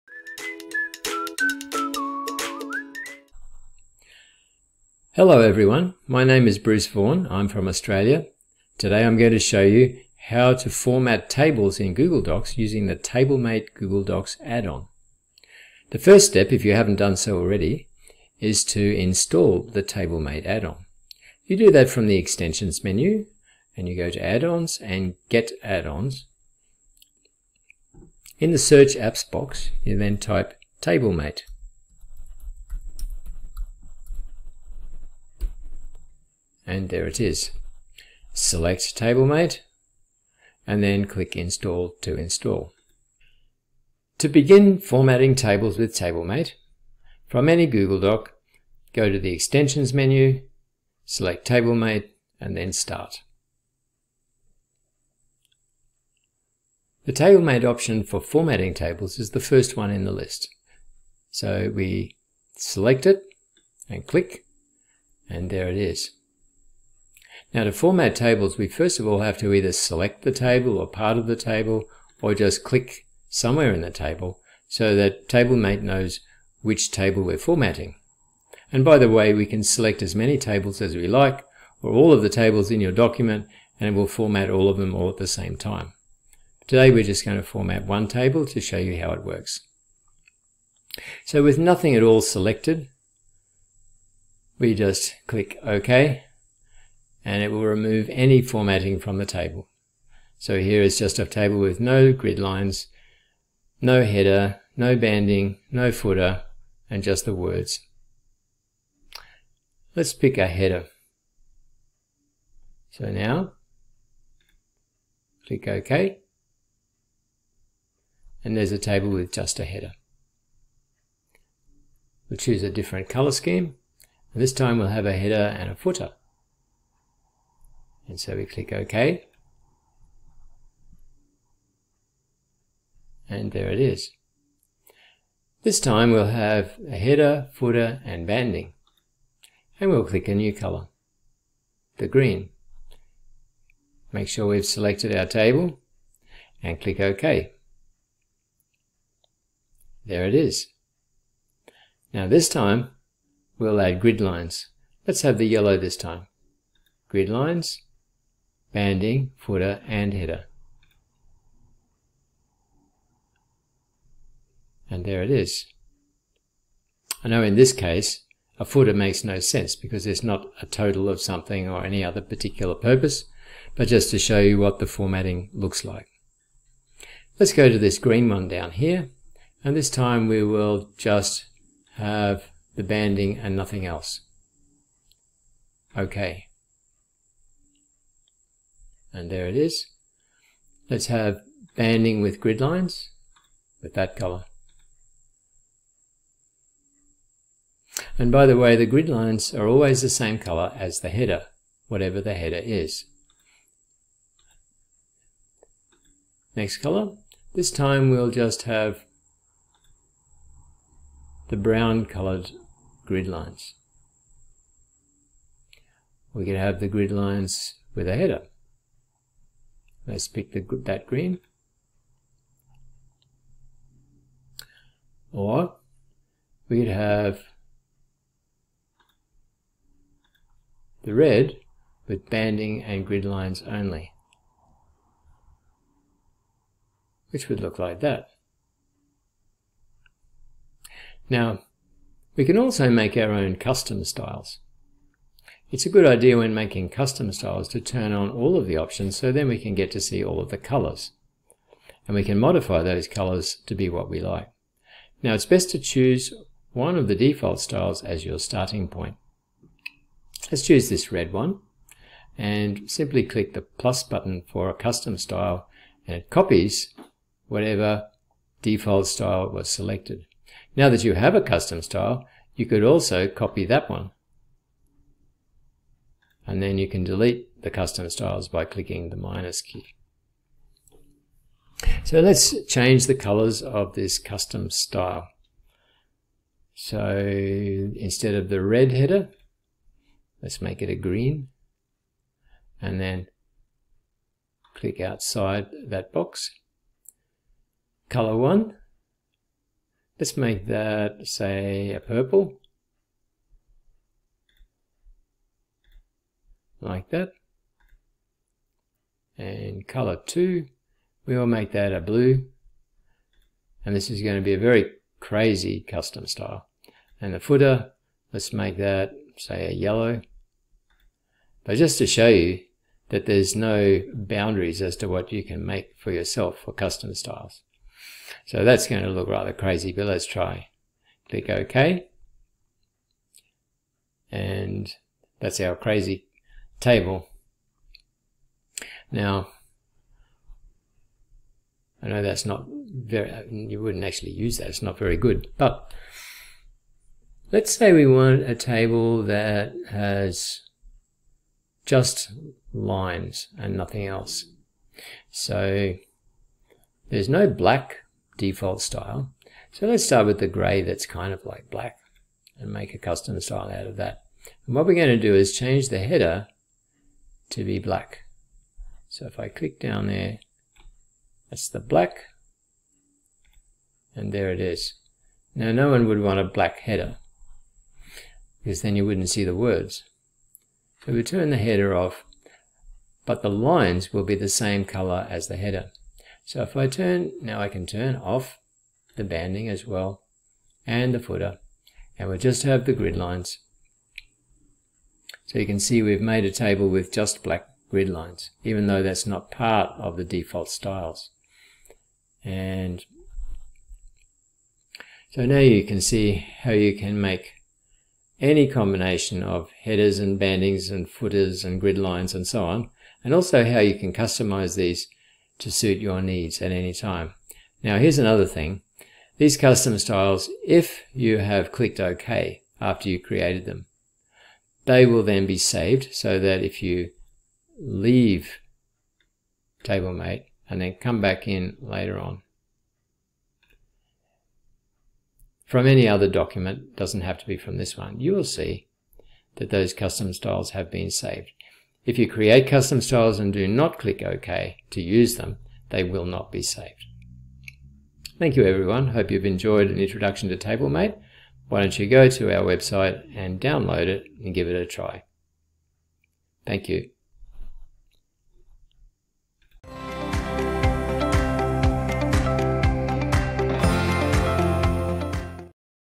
Google Docs – Format Tables
Format tables in Google Docs using the TableMate add-on. An almost unlimited variety of built-in styles and colours are provided. Also you can easily create your own custom styles.